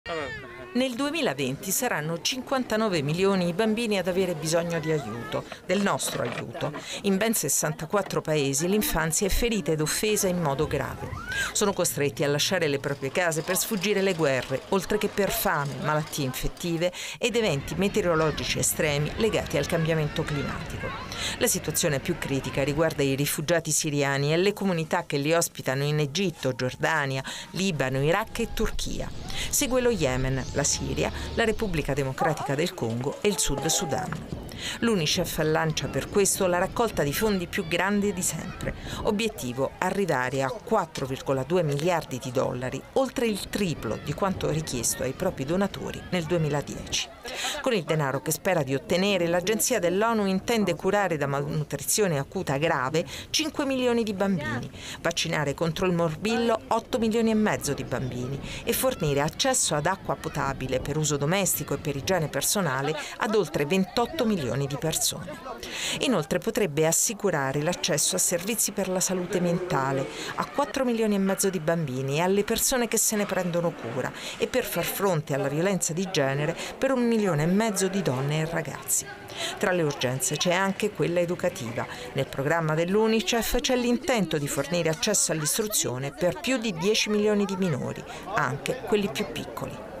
Unicef, 59 milioni di bambini avranno bisogno di aiuto nel 2020
Un bambino su 4 sul pianeta vive in paesi in guerra o colpiti da disastri naturali. Sono decine di milioni quelli in condizioni di estremo bisogno a causa dei conflitti, della fame, delle malattie.
Servizio di Clara Iatosti